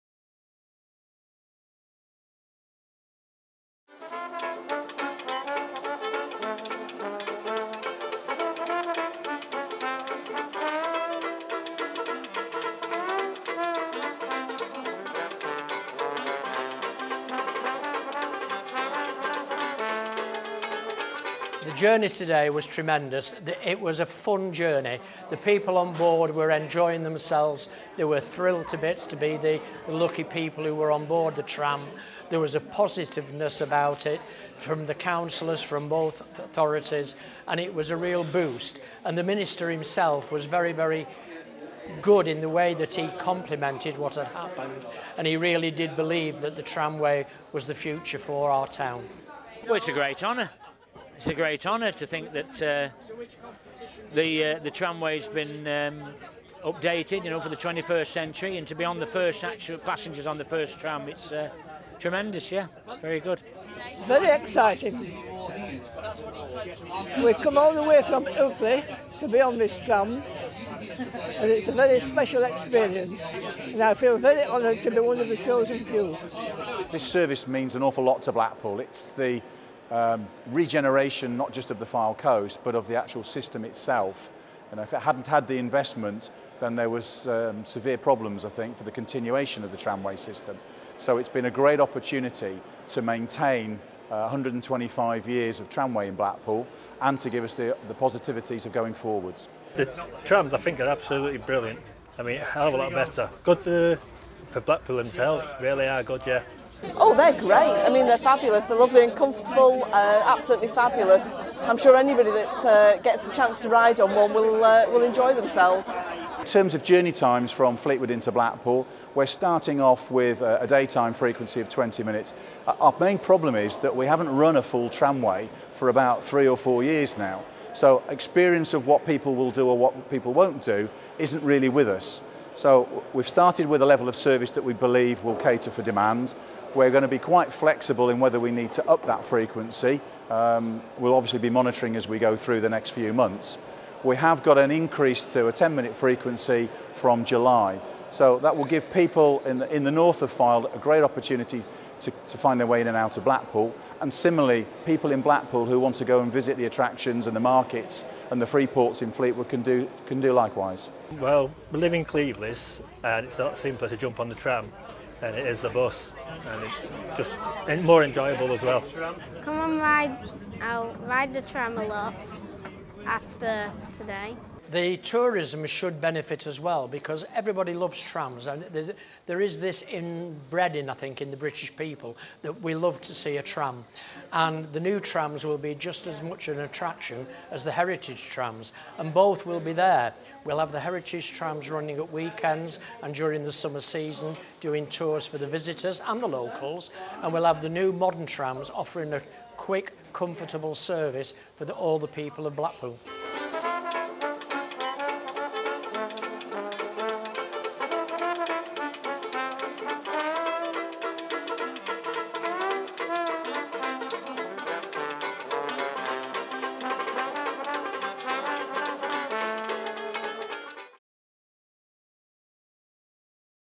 New Blackpool to Fleetwood tramway launches
Blackpool's new tramway has officially opened.

to see a photo gallery from the event.

The four year project to replace 11km of track, create a new state-of-the-art depot and introduce 16 new trams is set to launch after a £100 million investment. Funding came from Department for Transport, Blackpool Council, Lancashire County Council and INTERREG IVB North West Europe Programme.

Transport Minister Norman Baker MP officially cut the ribbon to mark the tramway open before 40 lucky 'golden ticket' competition winners boarded for the first passenger journey to Fleetwood.

The first passenger journey set off from the Blackpool Tower tram stop, accompanied by a tour guide and included a short stop at Cleveleys, before arriving at Fleetwood for a welcome party.